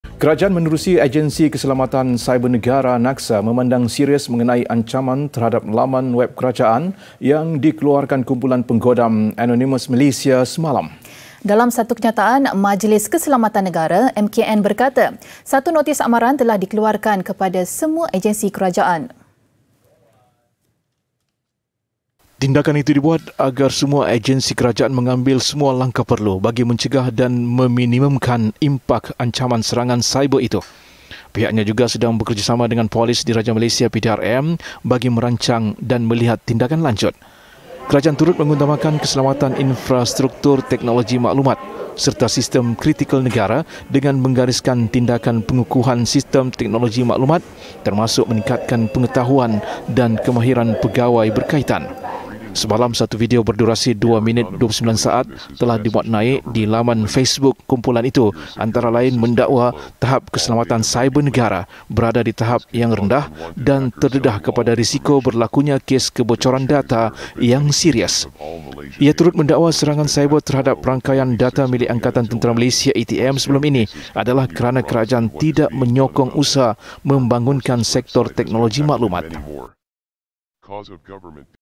ANONYMOUS MALAYSIA | MKN Keluar Notis Amaran, Kerjasama Dengan PDRM
Kerajaan menerusi Agensi Keselamatan Siber Negara (NACSA), memandang serius mengenai ancaman terhadap laman web kerajaan yang dikeluarkan kumpulan penggodam 'Anonymous Malaysia' semalam.